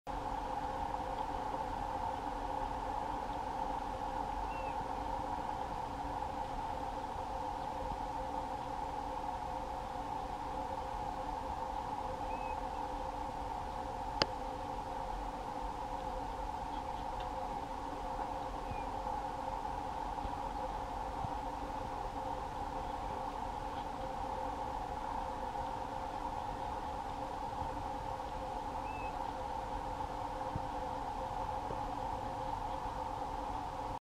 Dal grande Nord alla nostra Murgia / Pivieri dorati
Uno stormo di Pivieri dorati nel Parco Nazionale dell'alta Murgia.